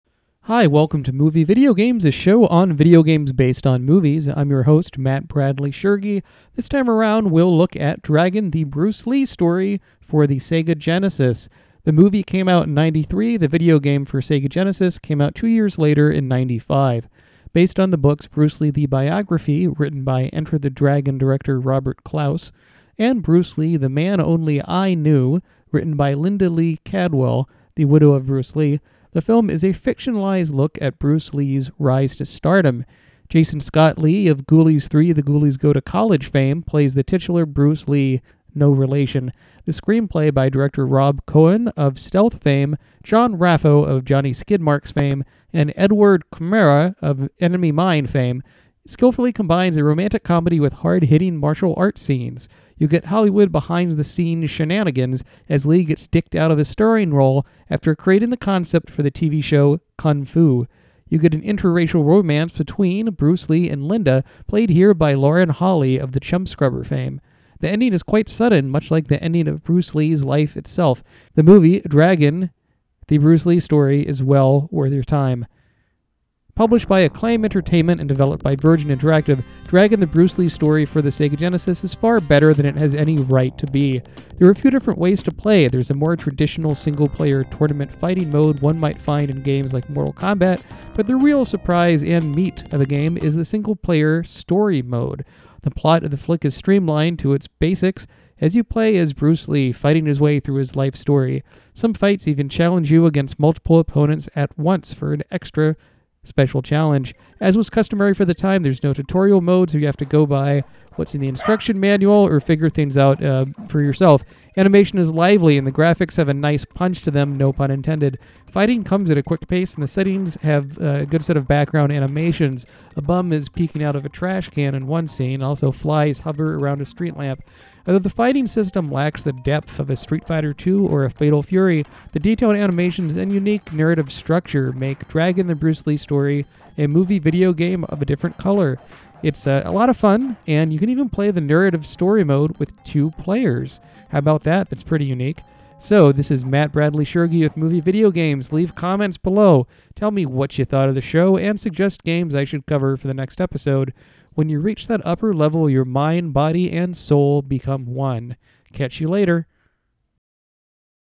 Dragon: The Bruce Lee Story: Movie Video Games
Mat Bradley-Tschirgi takes a look at Dragon: The Bruce Lee Story fir the Sega Genesis in this episode of Movie Video Games.

SCRIPT:

Hi, welcome to Movie Video Games, a show on video games based on movies. I’m your host, Mat Bradley-Tschirgi.

This time around we’ll look at Dragon: The Bruce Lee Story for the Sega Genesis.

The film came out in 1993 and the video game for the Sega Genesis came out two years later in 1995. Based on the books Bruce Lee: The Biography  (written by Enter the Dragon director Robert Clouse) and Bruce Lee: The Man Only I Knew (written by Linda Lee Cadwell, widow of Bruce Lee), the film is a fictionalized look at Bruce Lee’s rise to stardom. Jason Scott Lee of Ghoulies 3: The Ghoulies Go To College fame plays the titular Bruce Lee (no relation). The screenplay by director Rob Cohen of Stealth fame, John Raffo of Johnny Skidmarks fame, and Edward Khmara of Enemy Mine fame, skillfully combines a romantic comedy with hard-hitting martial arts scenes. You get Hollywood behind-the-scenes shenanigans as Lee gets dicked out of a starring role after creating the concept for the TV series Kung Fu. You get an interracial romance between Bruce Lee and Linda, played here by Lauren Holly of The Chumscrubber fame. The ending is sudden (much like the ending of the actor’s life itself), and the movie never quite exceeds the crackling excitement of the opening scenes in Hong Kong. Still, this is a rare biopic with lots of fun action. The movie Dragon: The Bruce Lee Story is well worth your time.

Published by Acclaim Entertainment and developed by Virgin Interactive, Dragon: The Bruce Lee Story for the Sega Genesis is far better than it has any right to be. There are a few ways to play. There’s the more traditional single-player tournament fighting mode one might find in games such as Mortal Kombat, but the real surprise of the game is the single-player mode. The plot of the flick is streamlined to its basics as you play as Bruce Lee fighting his way through his life story. Some fights even challenge you against multiple opponents for an extra challenge. As was customary for the time, no tutorial mode is in sight. Animation is lively and the graphics have a nice punch to them. Fighting comes at a quick pace and the settings have a nice set of animations to them. A bum might be peeking out from a trashcan or flies will hover around a streetlamp.

Although the fighting system lacks the depth of a Street Fighter II or a Fatal Fury, the detailed animations and unique narrative make Dragon: The Bruce Lee Story a movie video game of a different color. It’s a lot of fun and you can even play the narrative story mode with two players!

This is Mat Bradley-Tschirgi with Movie Video Games. When you reach that upper level, your mind, body, and soul become one. Catch you later!